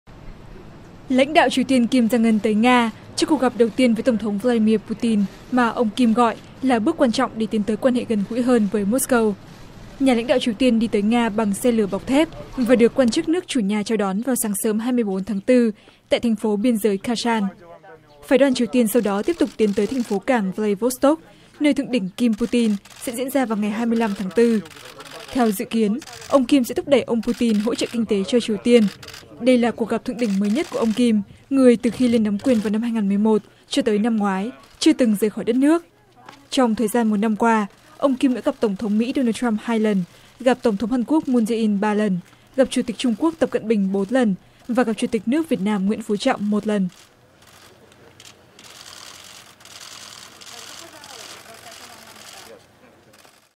Kim Jong Un tới Nga để siết chặt quan hệ với Moscow (VOA)
Tin tức Nếu không vào được VOA, xin các bạn hãy vào để vượt tường lửa. Lãnh đạo Triều Tiên Kim Jong-un tới Nga cho cuộc gặp đầu tiên với Tổng thống Vladimir Putin mà ông Kim gọi là bước quan trọng để tiến tới quan hệ gần gũi hơn với Moscow.

Nhà lãnh đạo Triều Tiên đi tới Nga bằng xe lửa bọc thép và được quan chức nước chủ nhà chào đón vào sáng sớm 24/4 tại thành phố biên giới Khasan.

Phái đoàn Triều Tiên sau đó tiếp tục tiến tới thành phố cảng Vladivostok, nơi thượng đỉnh Kim-Putin sẽ diễn ra vào ngày 25/4.

Theo dự kiến, ông Kim sẽ thúc đẩy ông Putin hỗ trợ kinh tế cho Triều Tiên. Đây là cuộc gặp thượng đỉnh mới nhất của ông Kim, người từ khi lên nắm quyền vào năm 2011 cho tới năm ngoái chưa từng rời khỏi đất nước. Trong thời gian một năm qua, ông Kim đã gặp Tổng thống Mỹ Donald Trump hai lần, gặp Tổng thống Hàn Quốc Moon Jae-in ba lần, gặp Chủ tịch Trung Quốc Tập Cận Bình bốn lần, và gặp Chủ tịch nước Việt Nam Nguyễn Phú Trọng một lần.